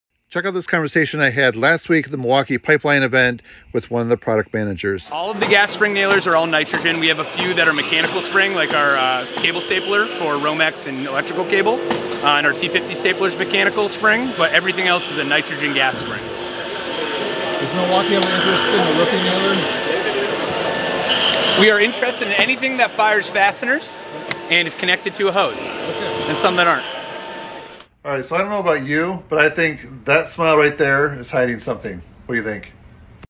What About A Milwaukee Roofing Nailer?
While I was at the Milwaukee 2023 PIPELINE Event, I was talking with one of their product managers about their different nailer technology and brought up the possibility of a M18 Roofing Nailer.  What do you think?  Does it look like they plan on introducing one soon?